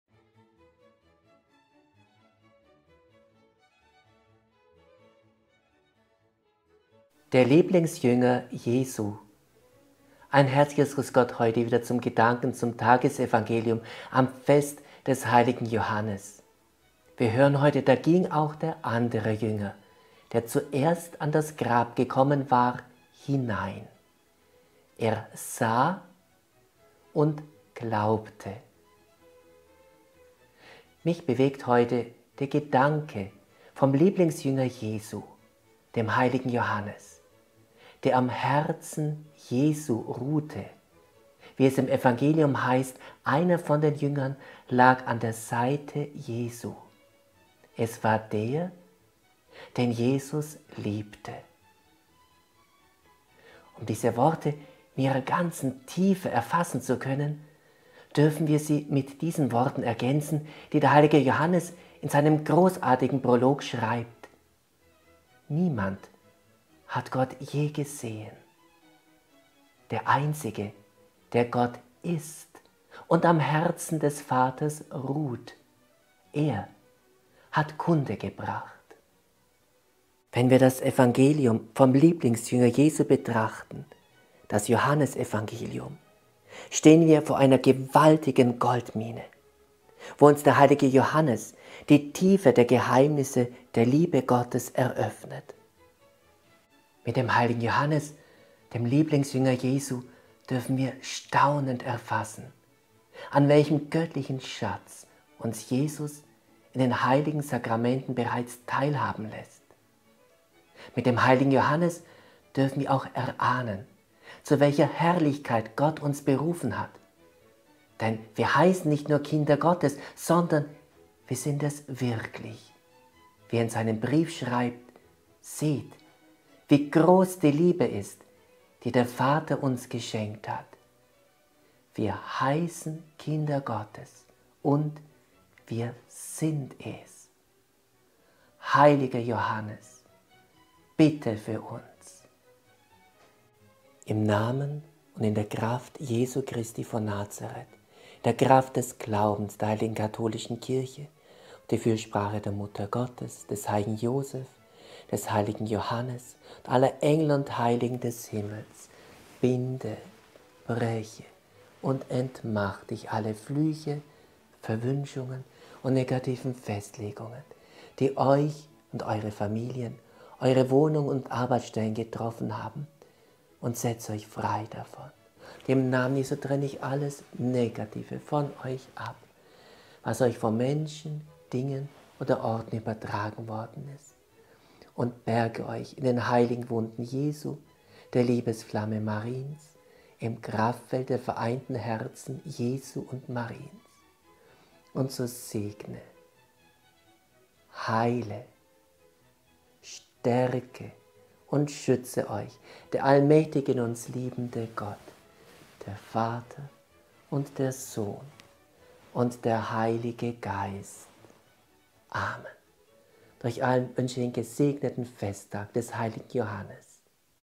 Der Lieblingsjünger Jesu
"Da ging auch der andere Jünger, der zuerst an das Grab gekommen war, hinein; er sah und glaubte." (Joh 20,8)

Mich bewegt heute der Gedanken vom Lieblingsjünger Jesu, den hl. Johannes, der am Herzen Jesu ruhte, wie es im Evangelium heißt: "Einer von den Jüngern lag an der Seite Jesu; es war der, den Jesus liebte." (Joh 13,23)

Um diese Worte in ihrer Tiefe zu erfassen, dürfen wir sie mit den Worten ergänzen, die der hl. Johannes in seinem großartigen Prolog schreibt: "Niemand hat Gott je gesehen. Der Einzige, der Gott ist und am Herzen des Vaters ruht, er hat Kunde gebracht." (Joh 1,18)

Ja, wenn wir das Evangelium vom Lieblingsjünger Jesu betrachten, stehen wir vor einer göttlichen Goldmine, wo uns der hl. Johannes die Tiefe der Geheimnisse der Liebe Gottes eröffnet.

Mit dem hl. Johannes dürfen wir staunend erfassen, an welchem göttlichen Schatz uns Jesus in den heiligen Sakramenten teilhaben lässt. Mit dem hl. Johannes dürfen wir erahnen, zu welcher Herrlichkeit Gott uns berufen hat, denn wir heißen nicht nur Kinder Gottes, sondern wir sind es wirklich: "Seht, wie groß die Liebe ist, die der Vater uns geschenkt hat: Wir heißen Kinder Gottes, und wir sind es." (1 Joh 3,1)

Hl. Johannes! – Bitte für uns!